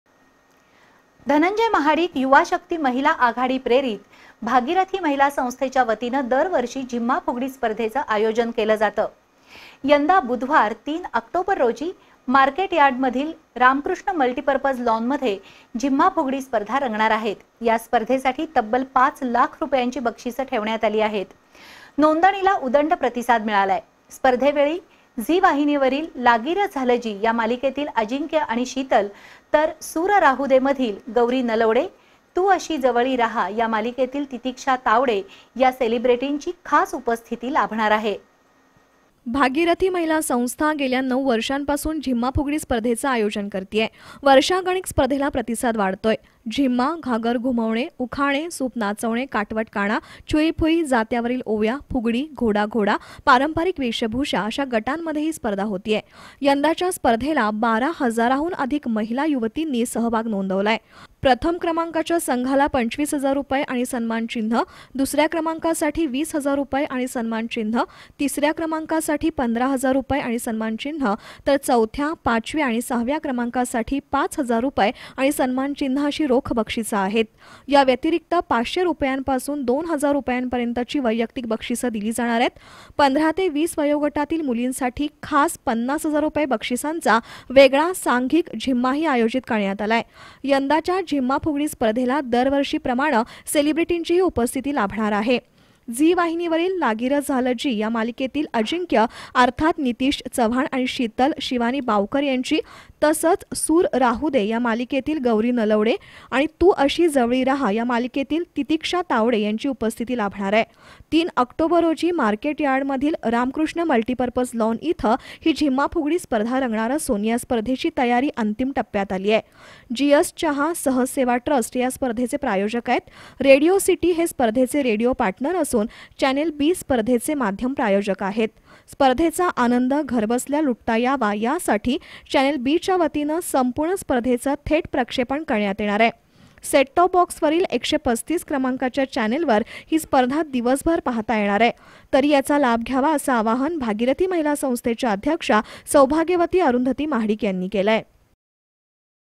भागीरथी महिला संस्थेच्या वतीनं ३ ऑक्टोबरला रंगणार झिम्मा-फुगडी स्पर्धा
भागीरथी महिला संस्थेच्या वतीनं कोल्हापुरातील रामकृष्ण मल्टिपर्पज लॉनवर  ३ ऑक्टोबरला रंगणार झिम्मा-फुगडी स्पर्धा
Channel B